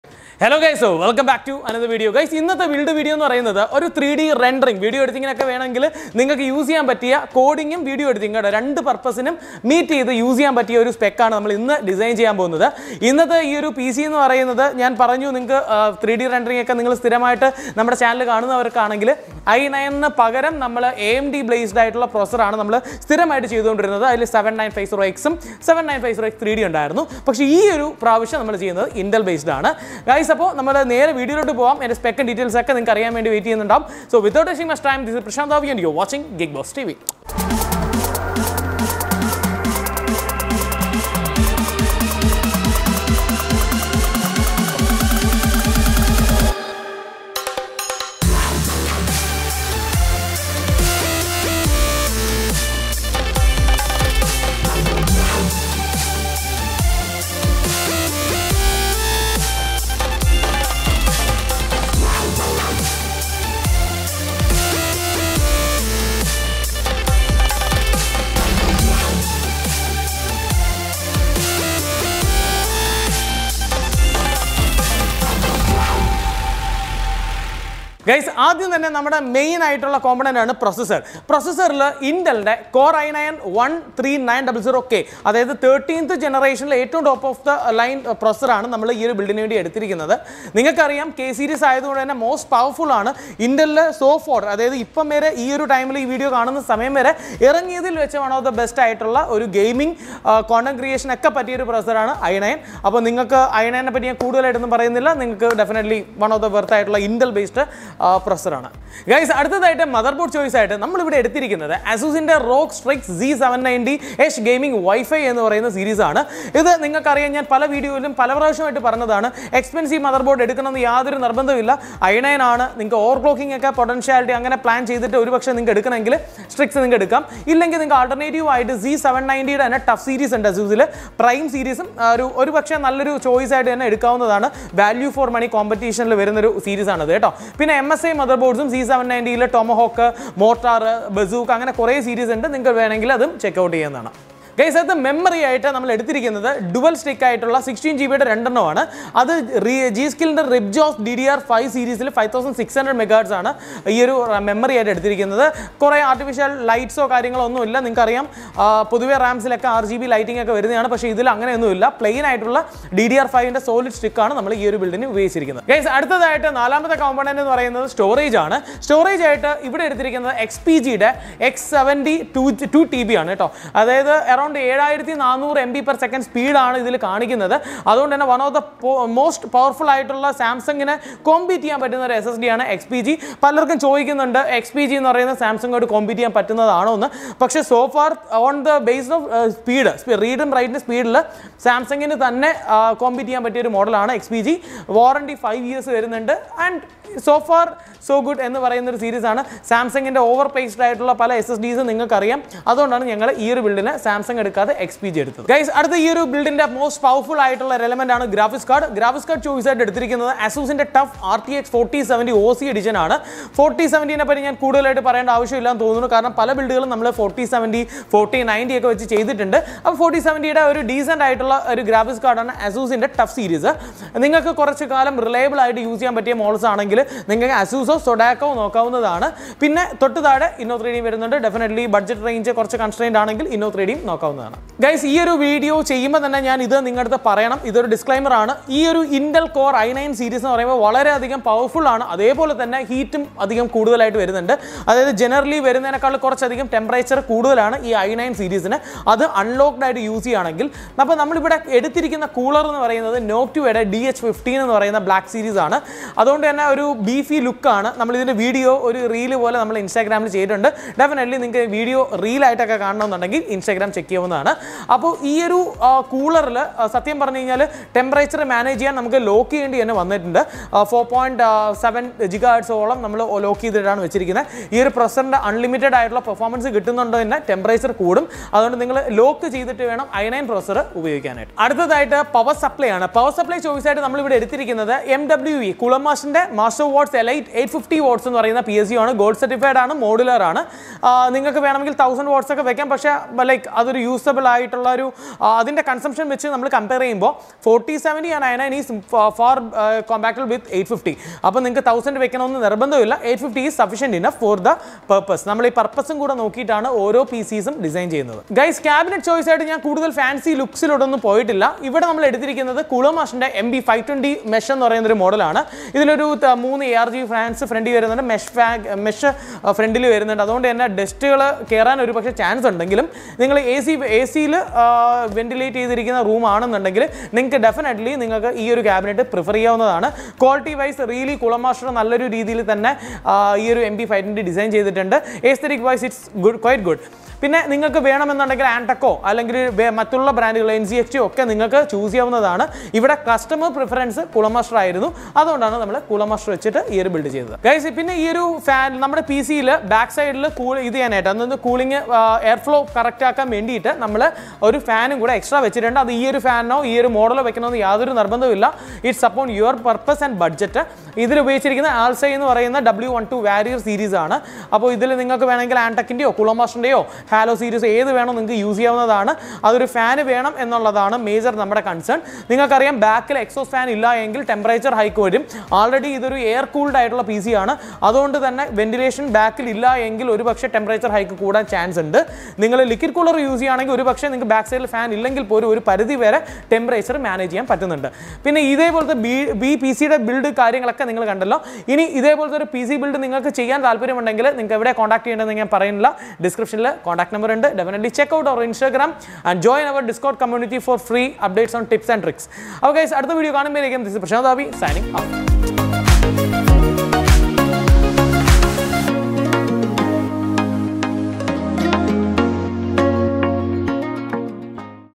INTEL PC BUILD for 3D Modelling and CODING Purpose | RTX 40 Series Build | 2023
Thanks for the Support Guys, Without you we are not going to Survive the Battle.

" BE THE GAME CHANGER WITH GEEKBOZ "